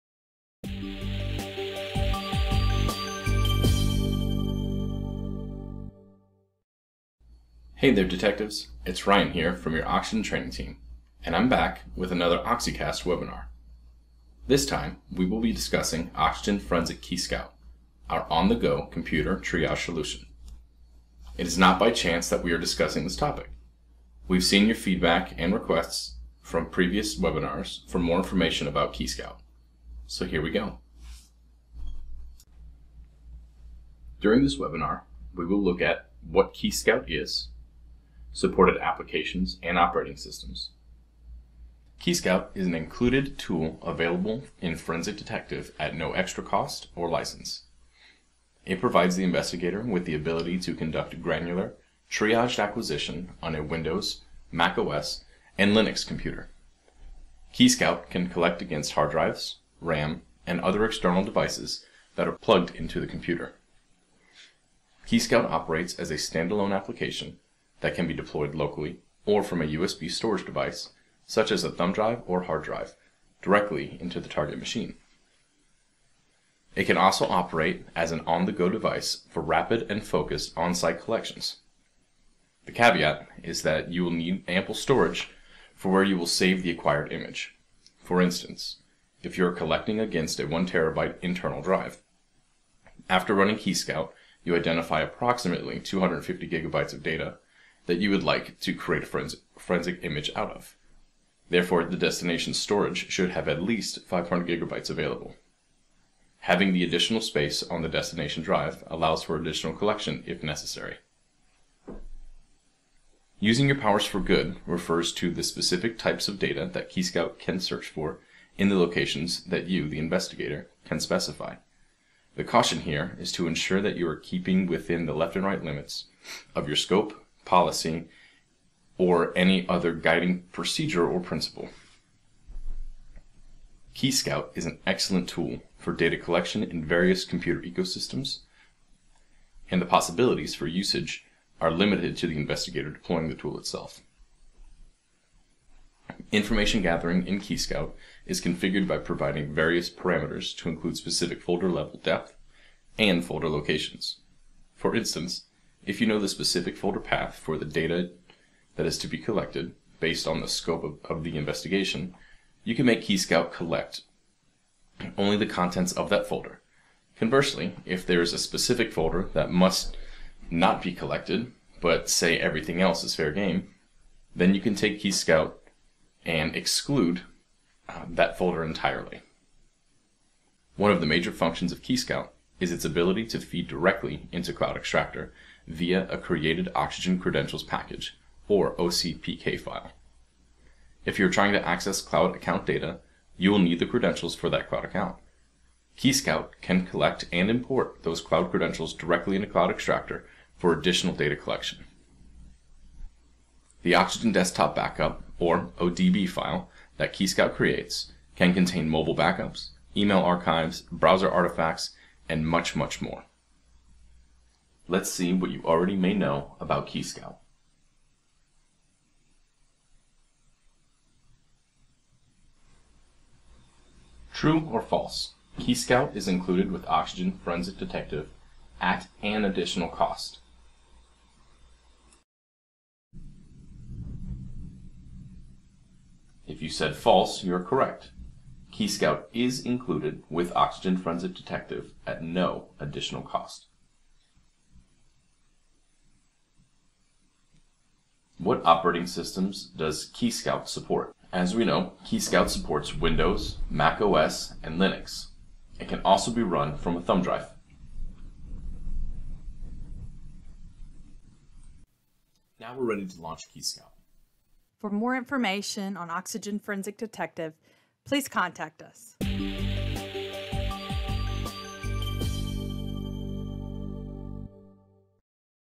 Get to Know KeyScout
We've seen your feedback and requests from previous webinars for more information about KeyScout.

During this webinar, we will look at what KeyScout is, supported applications, and operating systems.

Ryan, part of the Oxygen Training Team, helps further explore KeyScout and its capabilities.

Want to see more webinars? They’re Free.

Have an Oxygen Forensic® Detective license already, log into your LMS

00:00 Introduction
00:31 What is KeyScout?
01:54 Information Gathering
03:35 KeyScout FAQs
04:20 Conclusion